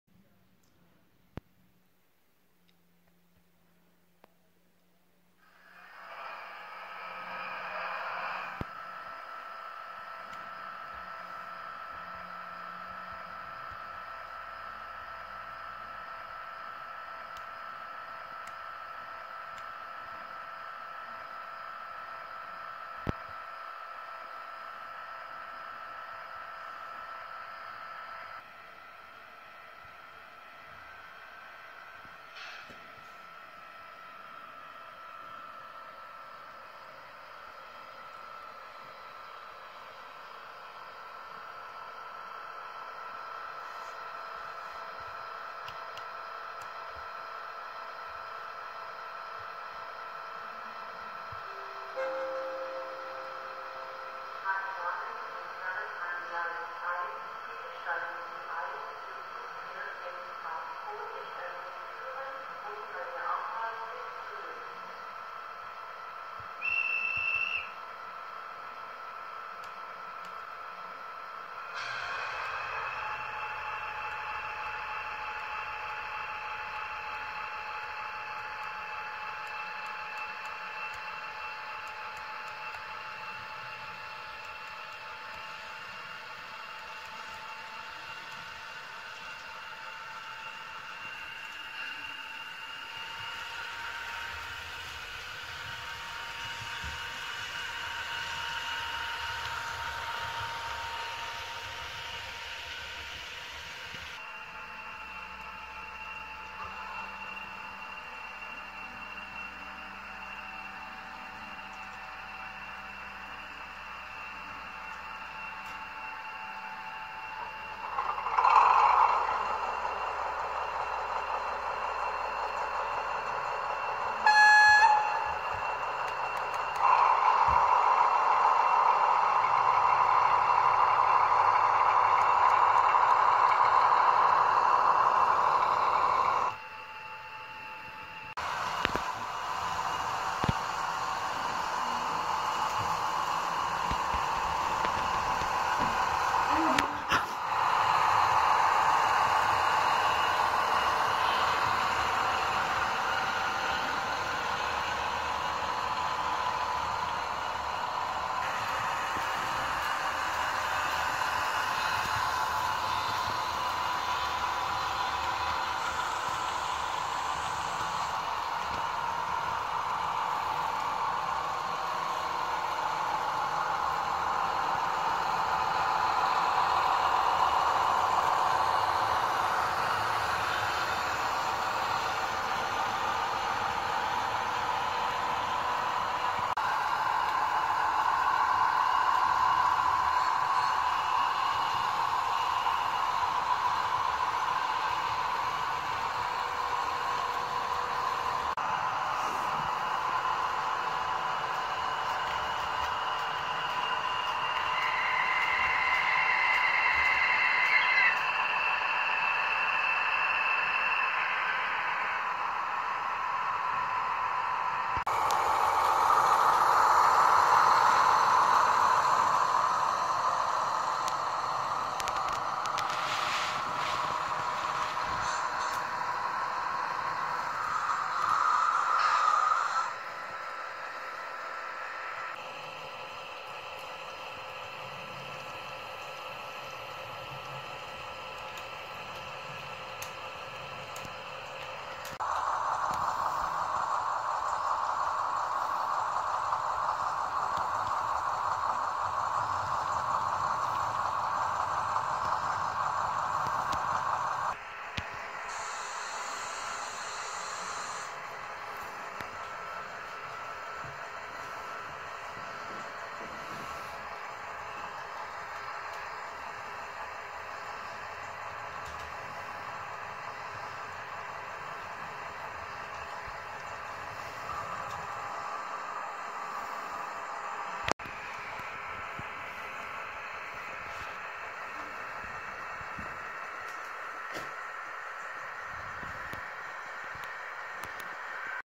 Märklin V160 026 29820 mit versteckten Sounds, 4255 und 4256 mit ESU 50708 und 2855 mit ESU Sound
Märklin v160 026 aus 29820 mit aktivierten versteckten Sounds ( Pfiff und Bahnsteigansage), Halterplatte getauscht und Schlussleuchten schaltbar gesteuert

Märklin 4255 und 4256 auf Kurzkupplung umgebaut, gealtert und mit Fahrgästen besetzt. Desweiteren haben sie ESU Innenbeleuchtung 50708 und Puffer 50710 erhalten mit beidseitig schaltbaren Schlusslicht. Dadurch sind beide Wagen autark und können in jeder Konstellation fahren.

Märklin 2855 mit nachgerüstetem Schlusslicht beidseitig schaltbar, Führerstandsbeleuchtung und ESU v5.0 Sound.

Märklin DHG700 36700 original mit Exact Train 20414 G-Wagen und Märklin Rungenwagen. Dieser ist Artitec Rollen beladen.